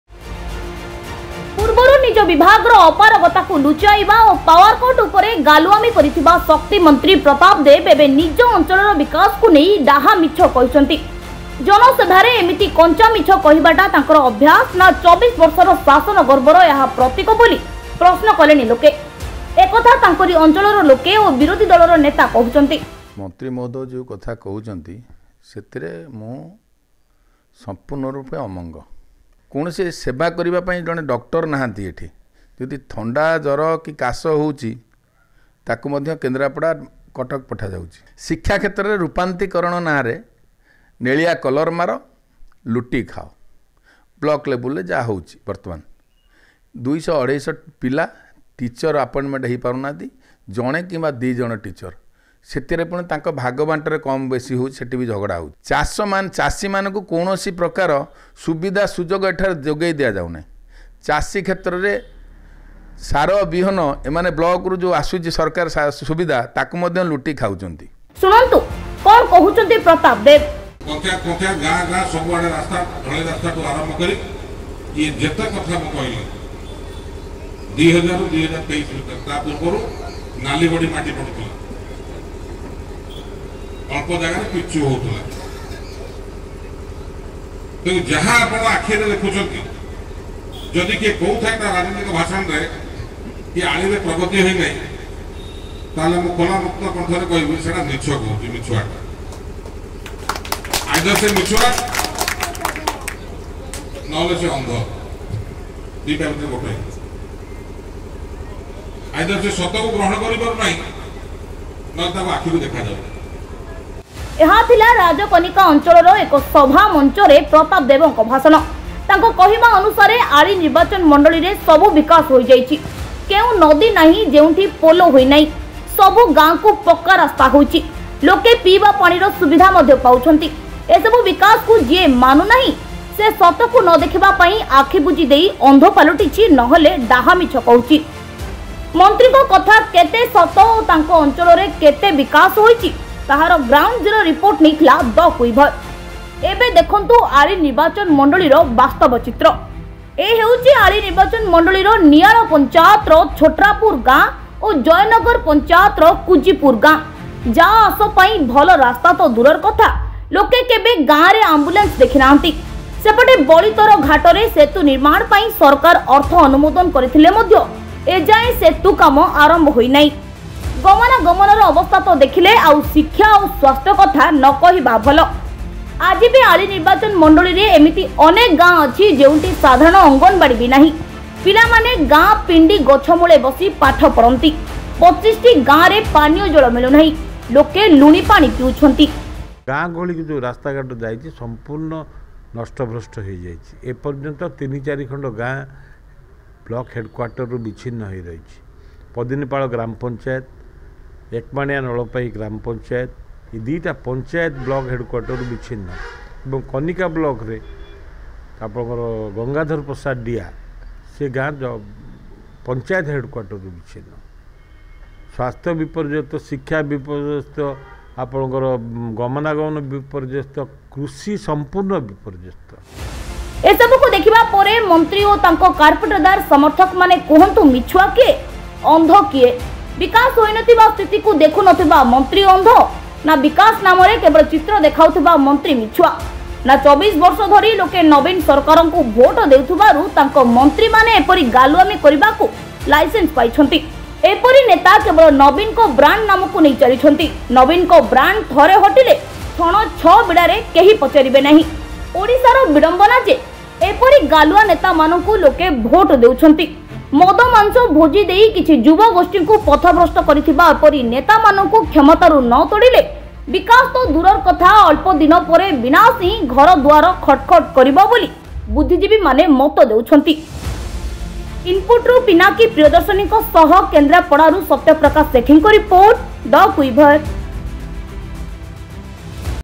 ଭାଷଣରେ ପ୍ରଦାପ ଦେବଙ୍କ ବିକାଶର ଚିତ୍ର | Development in Pratap's Speech | THE Quiver
ପୂର୍ବରୁ ନିଜ ବିଭାଗର ଅପାରଗତାକୁ ଲୁଚାଇବା ଓ ପାୱାର କଟ୍ ଉପରେ ଗାଲୁଆମୀ କରିଥିବା ଶକ୍ତିମନ୍ତ୍ରୀ ପ୍ରତାପ ଦେବ ଏବେ ନିଜ ଅଂଚଳର ବିକାଶକୁ ନେଇ ଡାହା ମିଛ କହିଛନ୍ତି । ଜନସଭାରେ ଏମିତି କଂଚାମିଛ କହିବାଟା ତାଙ୍କର ଅଭ୍ୟାସ ନା ୨୪ବର୍ଷର ଶାସନ ଗର୍ବର ଏହା ପ୍ରତୀକ ବୋଲି ପ୍ରଶ୍ନ କଲେଣି ଲୋକେ । ଏକଥା ତାଙ୍କରି ଅଂଚଳର ଲୋକେ ଓ ବିରୋଧୀ ଦଳର ନେତା କହୁଛନ୍ତି ।